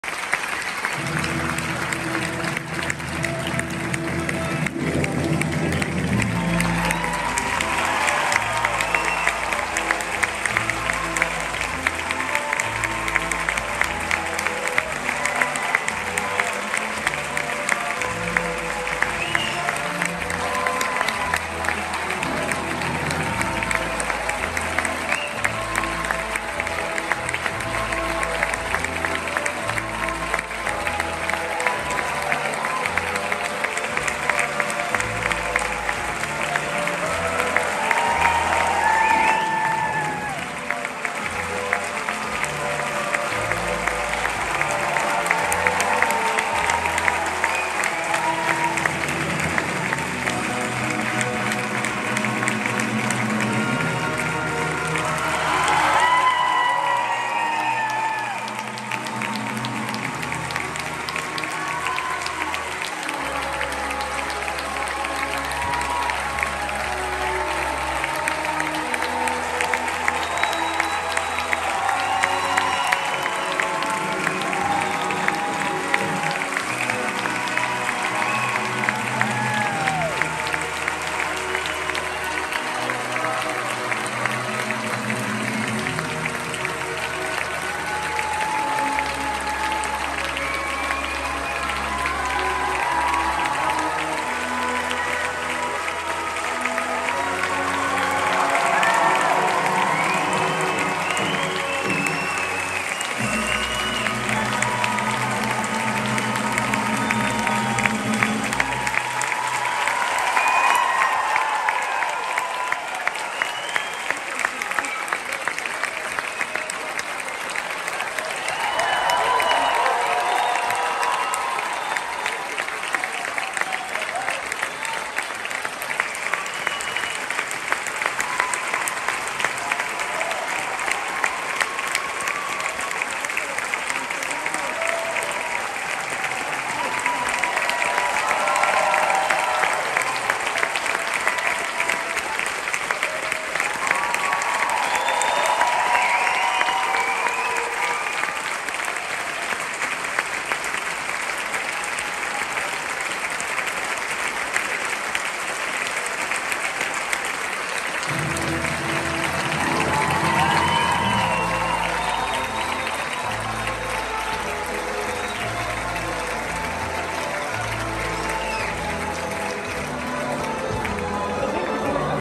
Birmingham Hippodrome Curtain Call Matthew Bourne Evening Wednesday 7February 24
Curtain call. Evening Wednesday 7 February 2024 The fantastic Edward Scissorhands ballet by Matthew Bourne New Adventures. At Birmingham Hippodrome. Well worth going to see and is on until this Saturday.